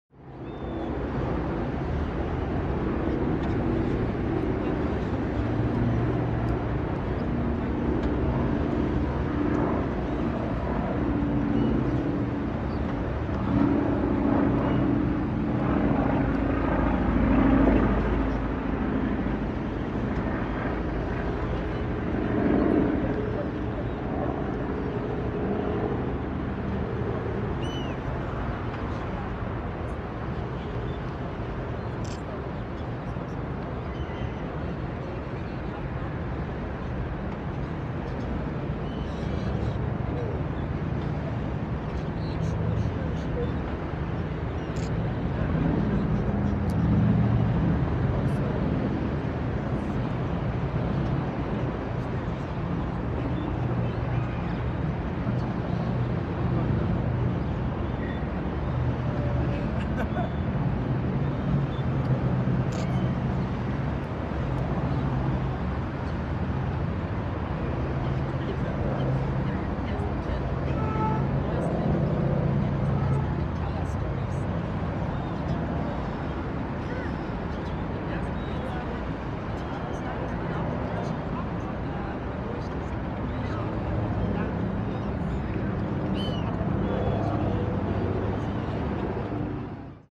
Hamburg, Binnenalster, Blick auf Alsterfontäne, Finnlandhaus - Full HD (1080p) Videobild
Full HD Video (1920x1080p).
Vollbildansicht mit 1080p wählen für alle Bilddetails.
Select full screen mode with 1080p for all image details.

Aufnahmedatum / Recording date:  10-2013

Kamera / Camera: Panasonic Lumix GH3
mit/with Rode Stereo VideoMic Pro

You-Tube upload quality:
Full HD 1920x1080p / 18-30 Mbit/s / 25fps